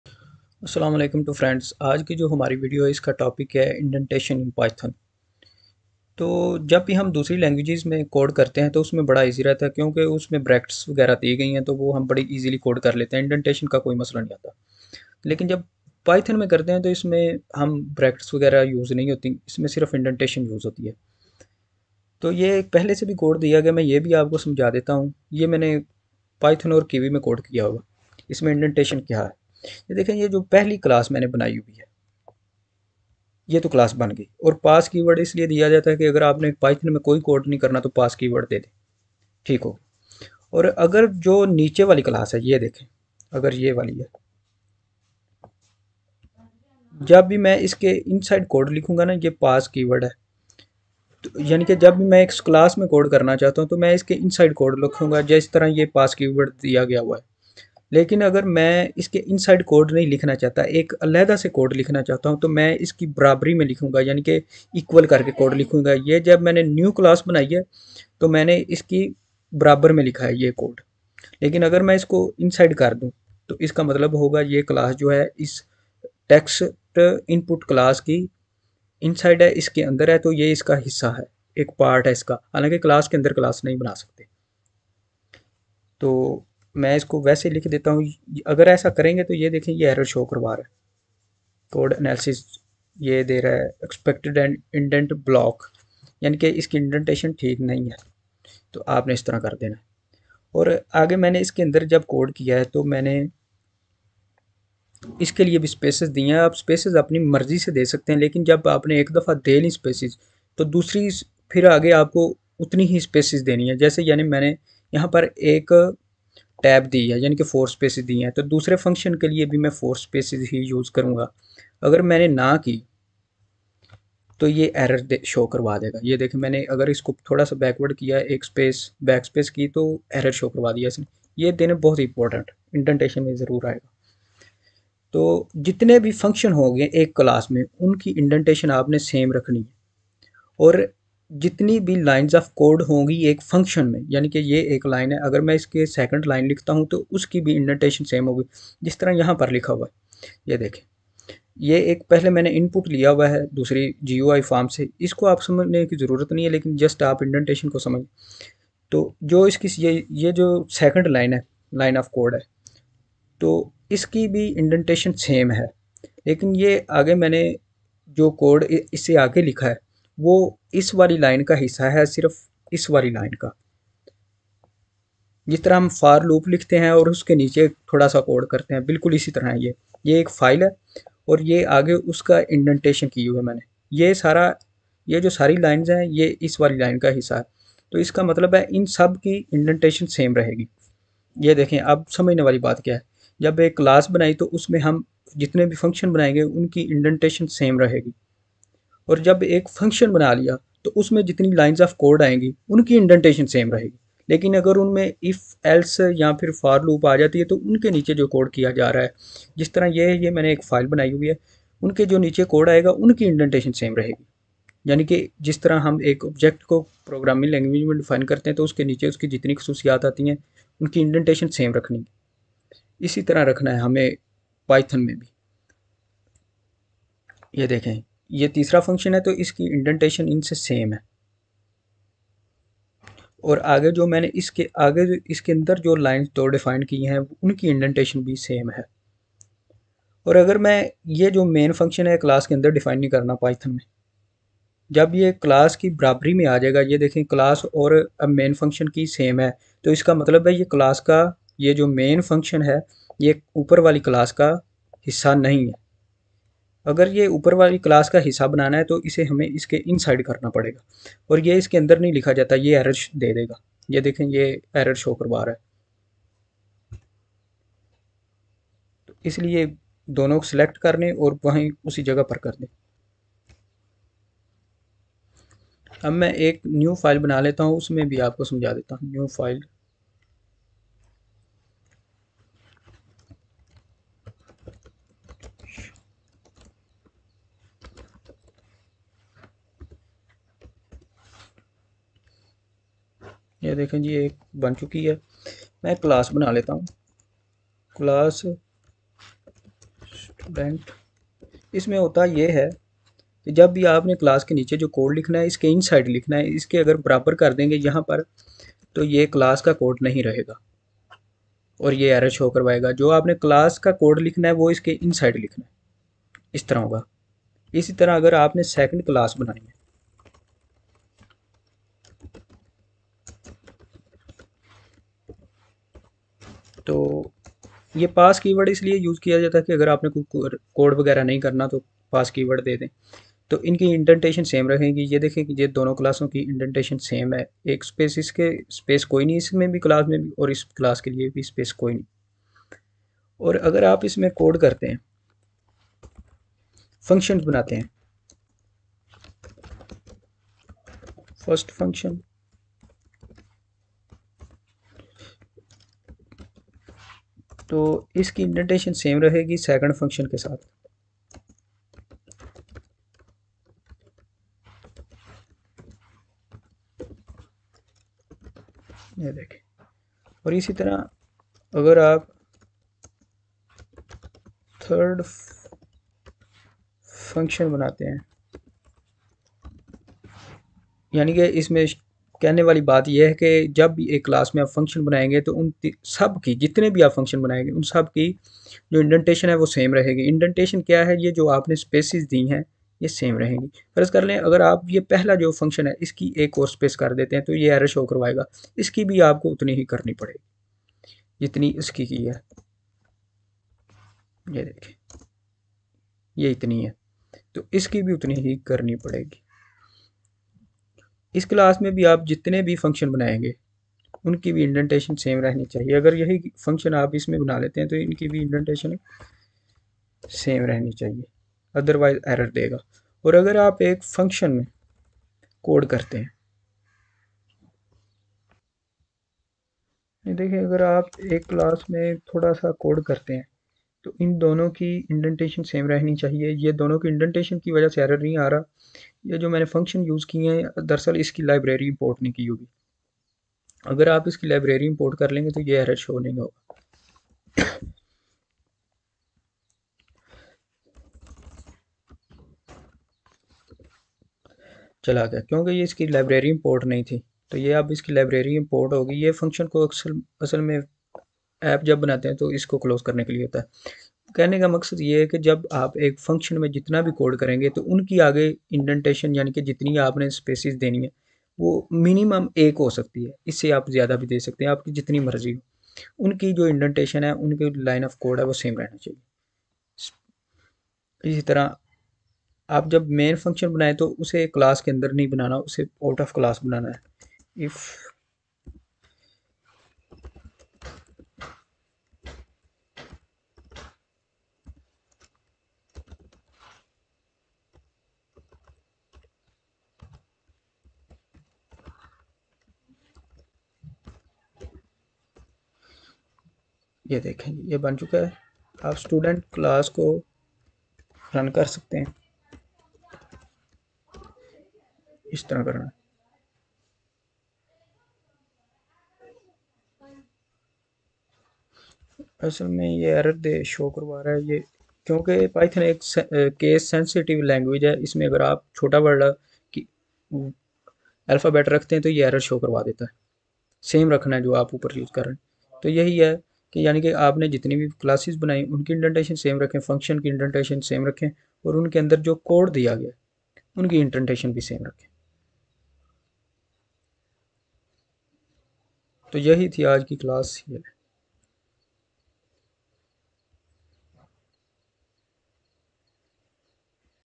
Indentation in Python
In this video we will learn how  to the indentation of python.